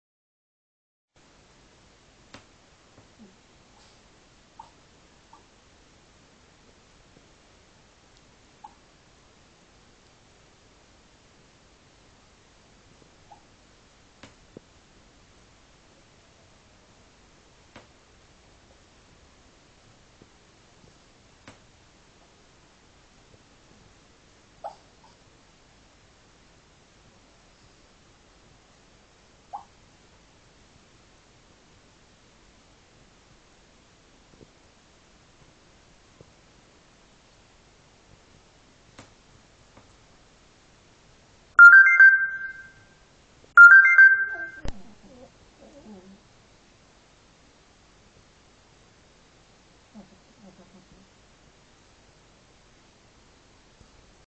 寝言？
キュンキュン・・キュンキュン・・・
時折こんな声を出しながら寝てるハナ。寝言なのかなぁ？^^

※スマホ撮影してる最中にメール受信してしまい、
40秒辺りから着信音が2回鳴りますので音量ご注意下さいませ^^;
ハナは着信音が鳴った瞬間から寝言がいびきに変わりますｗｗ